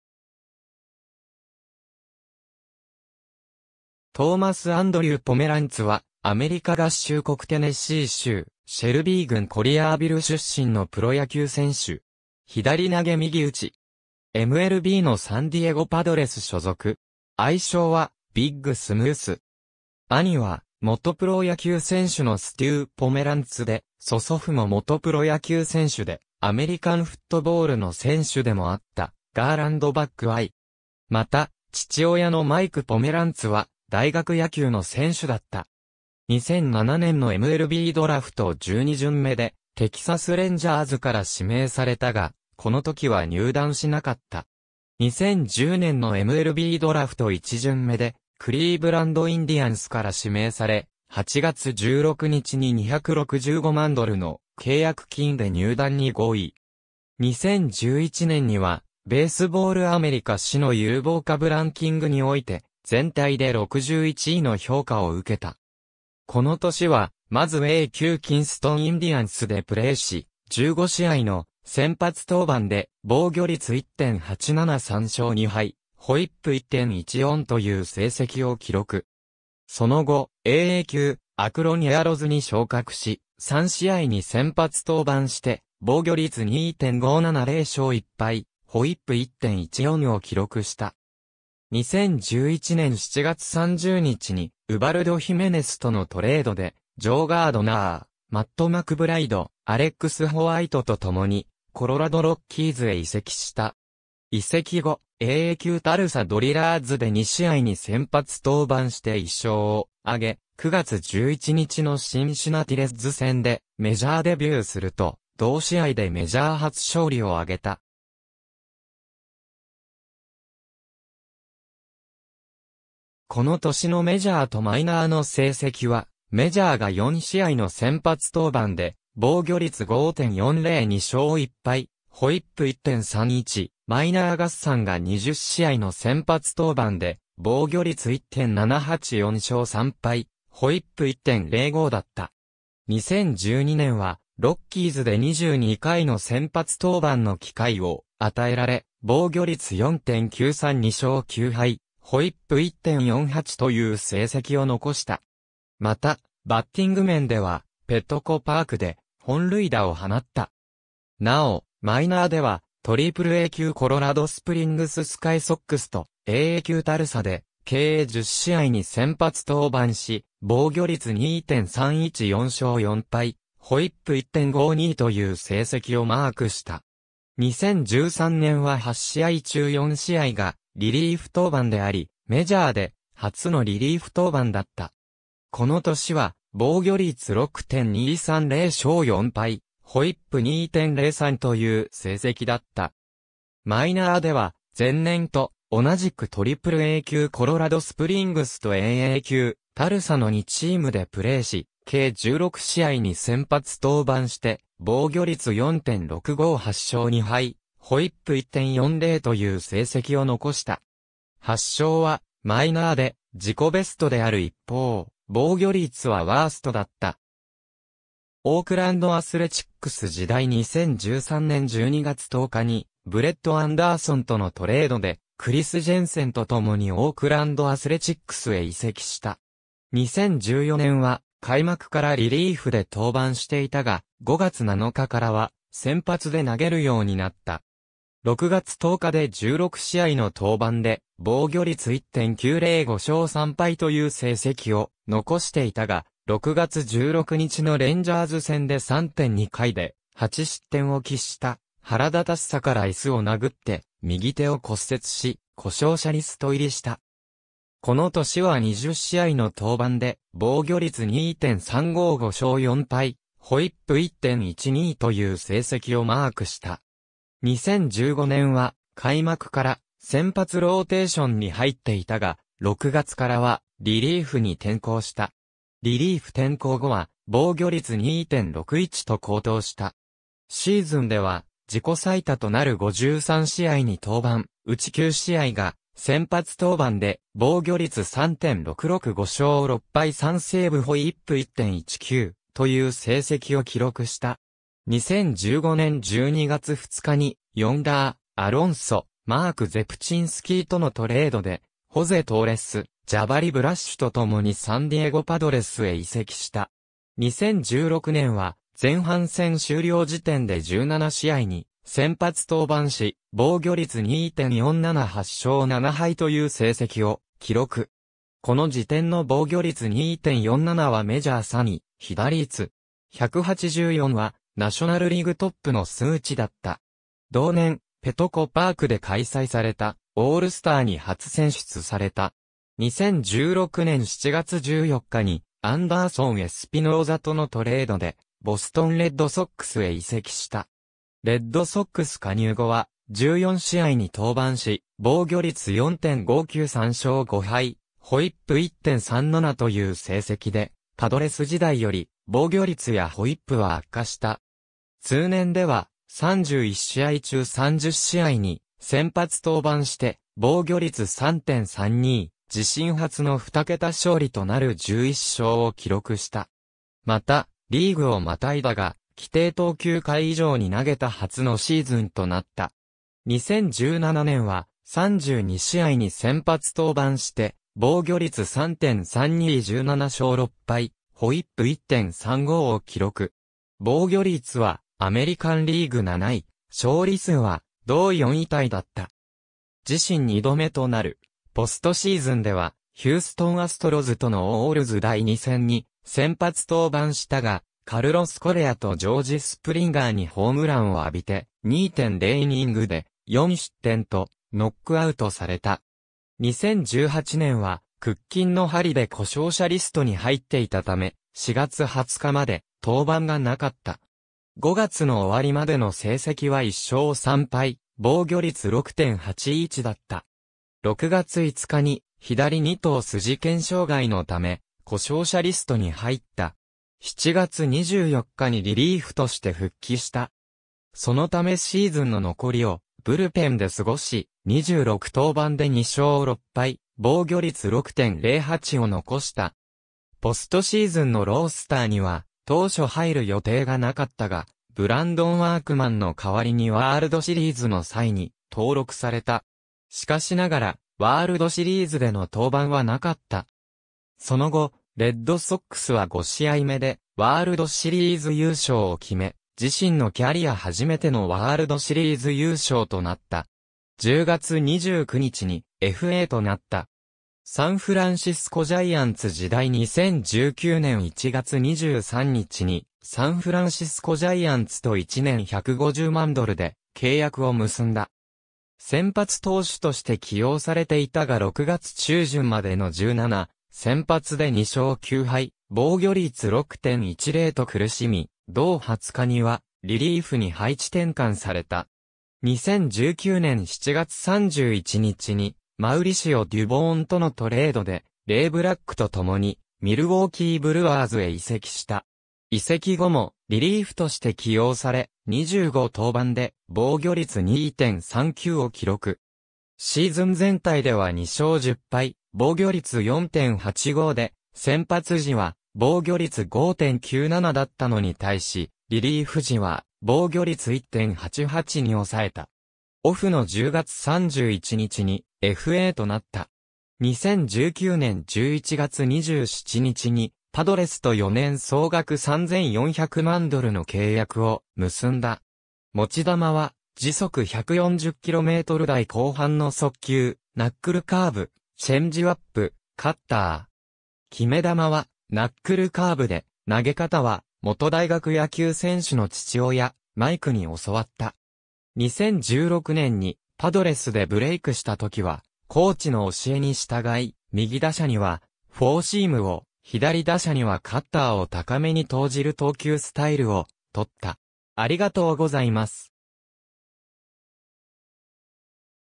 ドリュー・ポメランツ
コロラド・ロッキーズ傘下の選手
オークランド・アスレチックス傘下の選手
トーマス・アンドリュー・ポメランツ (Thomas Andrew Pomeranz, 1988年11月22日 - ) は、アメリカ合衆国テネシー州シェルビー郡コリアービル出身のプロ野球選手 (投手) 。
左投右打。
MLBのサンディエゴ・パドレス 所属。
愛称はビッグ・スムース。
兄は元プロ野球選手のステュー・ポメランツで、曽祖父も元プロ野球選手でアメリカンフットボールの選手でもあったガーランド・バックアイ。
また、父親のマイク・ポメランツは大学野球の選手だった。
2007年のMLBドラフト12巡目（全体380位）でテキサス・レンジャーズから指名されたが、この時は入団しなかった。
2010年のMLBドラフト1巡目（全体5位）でクリーブランド・インディアンスから指名され、8月16日に265万ドルの契約金で入団に合意。
2011年には、「ベースボール・アメリカ」誌の有望株ランキングにおいて、全体で61位の評価を受けた。
この年は、まずA+級キンストン・インディアンスでプレーし、15試合の先発登板で防御率1.87・3勝2敗・WHIP1.14という成績を記録。
その後、AA級アクロン・エアロズに昇格し、3試合に先発登板して防御率2.57・0勝1敗・WHIP1.14を記録した。
2011年7月30日にウバルド・ヒメネスとのトレードで、ジョー・ガードナー、マット・マクブライド、アレックス・ホワイトと共にコロラド・ロッキーズへ移籍した (実際にポメランツが移籍したのは8月16日) 。
移籍後、AA級タルサ・ドリラーズで2試合に先発登板して1勝を挙げ、9月11日のシンシナティ・レッズ戦でメジャーデビューすると、同試合でメジャー初勝利を挙げた。
この年のメジャーとマイナーの成績は、メジャーが4試合の先発登板で防御率5.40・2勝1敗・WHIP1.31、マイナー合算が20試合の先発登板で防御率1.78・4勝3敗・WHIP1.05だった。
2012年は、ロッキーズで22回の先発登板の機会を与えられ、防御率4.93・2勝9敗・WHIP1.48という成績を残した。
また、バッティング面ではペトコ・パークで本塁打を放った。
なお、マイナーではAAA級コロラドスプリングス・スカイソックスとAA級タルサで計10試合に先発登板し、防御率2.31・4勝4敗・WHIP1.52という成績をマークした。
2013年は8試合中4試合がリリーフ登板であり、メジャーで初のリリーフ登板だった。
この年は、防御率6.23・0勝4敗・WHIP2.03という成績だった。
マイナーでは、前年と同じくAAA級コロラドスプリングスとAA級タルサの2チームでプレーし、計16試合に先発登板して防御率4.65・8勝2敗・WHIP1.40という成績を残した。
8勝は、マイナーで自己ベストである一方、防御率はワーストだった。
オークランド・アスレチックス時代 (2015年8月17日) 2013年12月10日にブレット・アンダーソンとのトレードで、クリス・ジェンセンと共にオークランド・アスレチックスへ移籍した。
2014年は、開幕からリリーフで登板していたが、5月7日からは先発で投げるようになった。
6月10日で16試合の登板で防御率1.90・5勝3敗という成績を残していたが、6月16日のレンジャーズ戦で3.2回で8失点を喫した腹立たしさから椅子を殴って右手を骨折し、故障者リスト入りした。
この年は20試合の登板（先発10試合）で、防御率2.35・5勝4敗・WHIP1.12という成績をマークした。
2015年は、開幕から先発ローテーションに入っていたが、6月からはリリーフに転向した。
リリーフ転向後は、防御率2.61と好投した。
シーズンでは自己最多となる53試合に登板・うち9試合が先発登板で、防御率3.66・5勝6敗3セーブ (セーブはメジャー初) ・WHIP1.19という成績を記録した。
2015年12月2日にヨンダー・アロンソ、マーク・ゼプチンスキーとのトレードで、ホゼ・トーレス、ジャバリ・ブラッシュ（12月10日に移籍）と共にサンディエゴ・パドレスへ移籍した。
2016年は、前半戦終了時点...